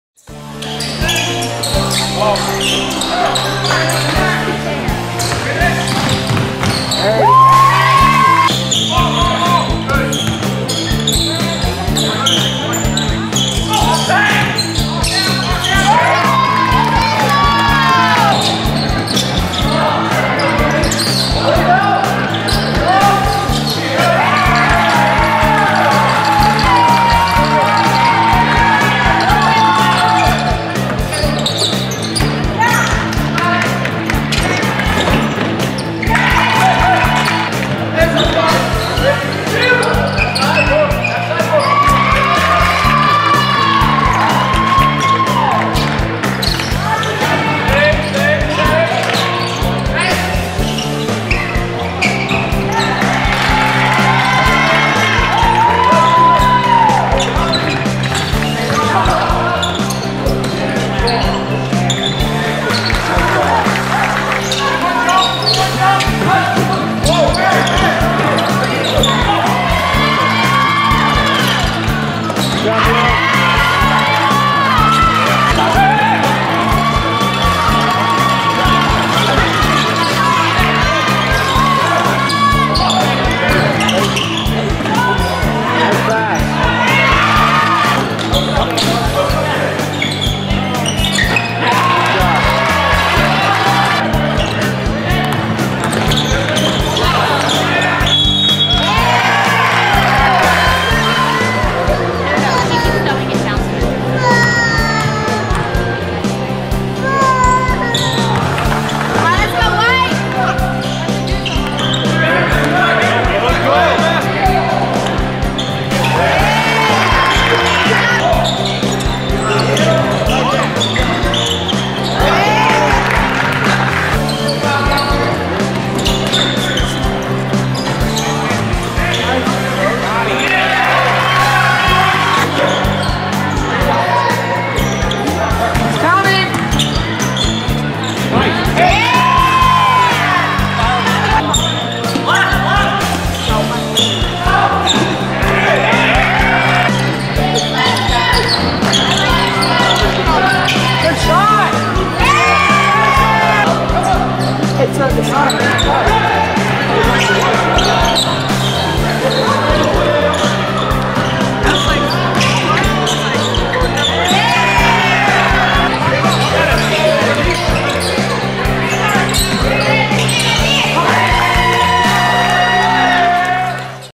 Milos Vasilic Basketball Promo Video
Milos Vasilic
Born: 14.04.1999
Height: 6,6" (199 cm)
Weight: 182 lbs (83 kg)
Wingspan: 6,8" (203 cm)
High school: Shenandoah Valley Christian Academy (Junior), VA